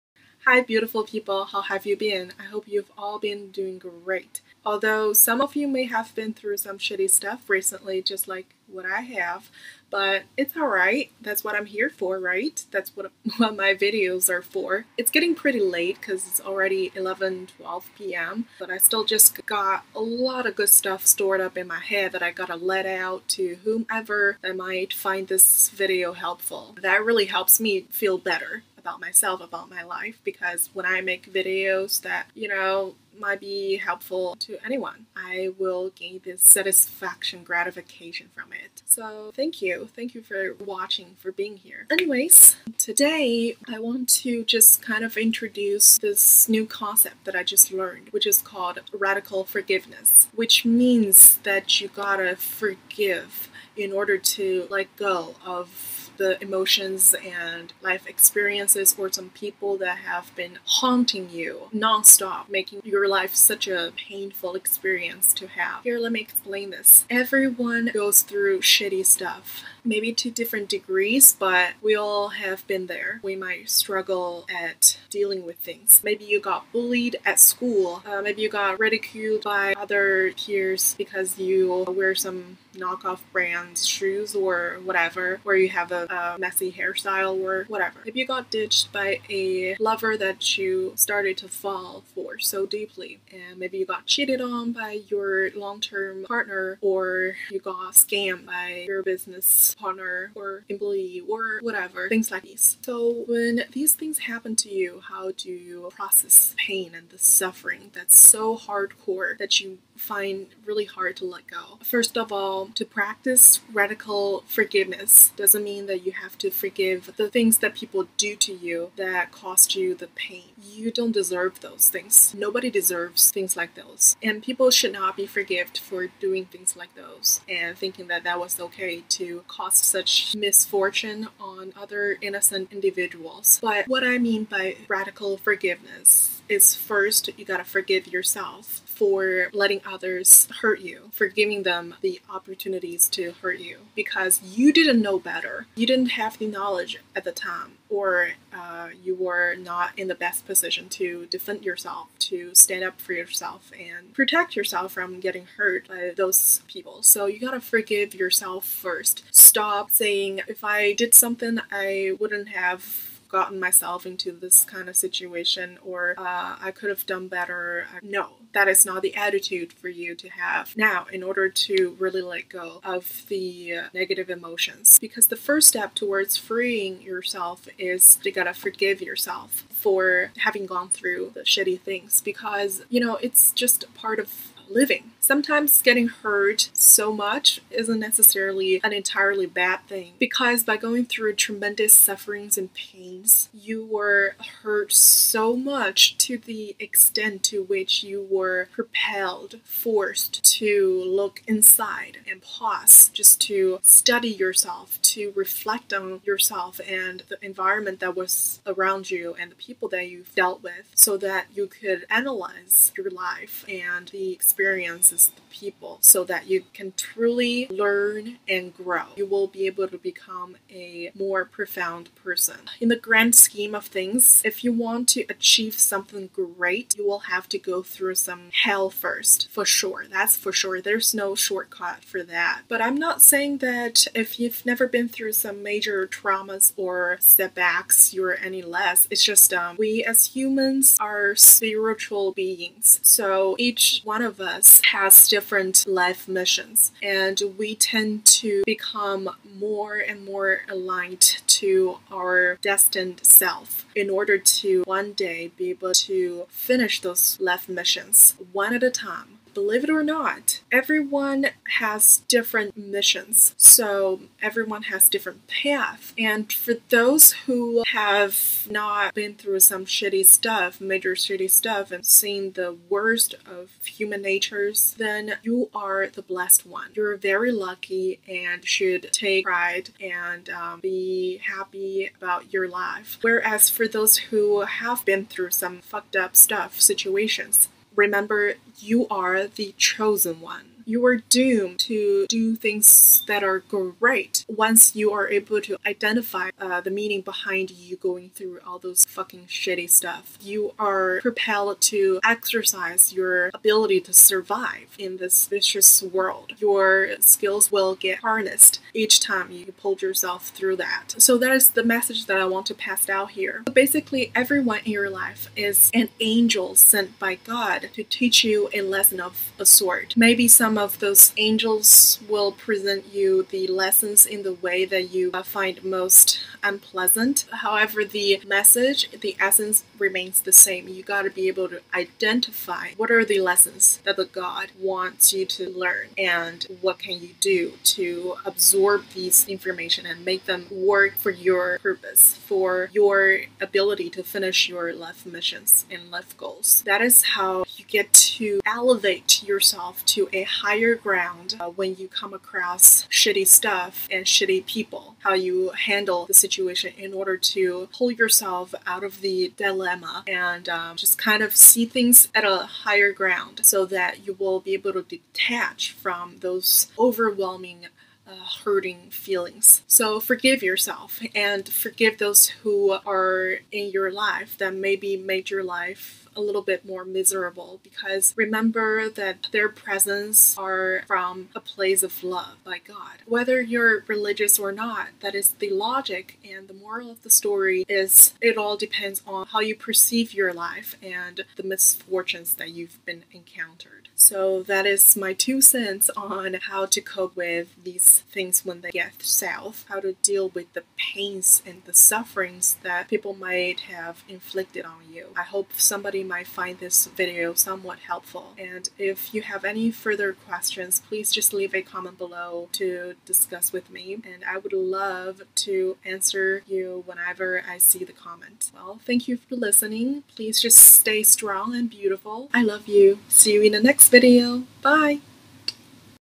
When you are feeling desperate and sad. What to do when you are hurt by others
I hope this video can be helpful to you.